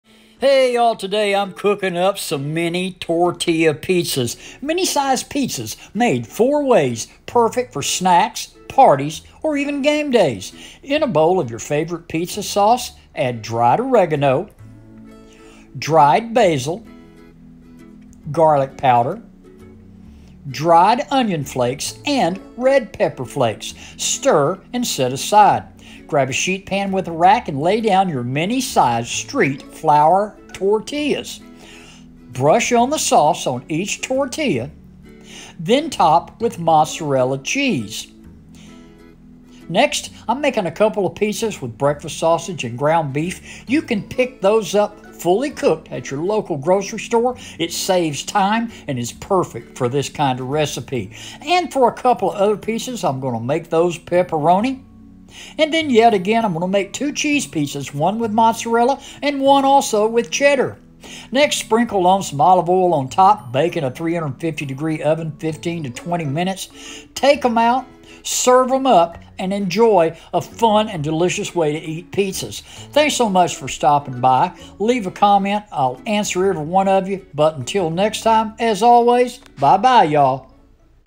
MINI TORTILLA PIZZA | Mini Pizzas Made 4 Ways
Easy mini tortilla pizza recipe perfect for snack time or as party appetizers. Mini tortilla pizzas made to your liking with toppings of your choice. These delicious single serve pizzas are made with homemade Italian seasoned pizza sauce topped with ground beef, pepperoni, sausage and mozzarella or cheddar cheeses.  Tasty pizza treats for kids and adults alike.

Servings: 6 mini pizzas

Shopping List:
(6) Mini street taco flour tortillas
2 cups pizza sauce
1/2 tsp. each dried oregano, dried basil, garlic powder, minced onions flakes, red pepper flakes.
Pinch salt
1 cup shredded mozzarella cheese
1/4 cup shredded cheddar cheese
(6) pepperoni rounds, two of which are roughly chopped
(2) tbsps. breakfast sausage bits
(2) tbsps. ground chuck beef
Sprinkle of olive oil

Directions:
Pre-heat oven to 350F.
In a bowl, add pizza sauce and seasonings. Stir.
Place tortillas on a rack over a sheet pan.
Brush each tortilla with sauce adding the toppings of your choice for each pizza.
Sprinkle the tops with olive oil and bake in oven for 5-7 minutes or until the cheese is bubbly and the crust is crispy.

License Background Eraser software: available through Apple App, Seller:  Tasnim AhmedFilmora.com/io  (Wonderland)
Thumbnail Image: Phonto, available from Apple Store
Thumbnail Image: Canva.com
Thumbnail Image: Remove.bg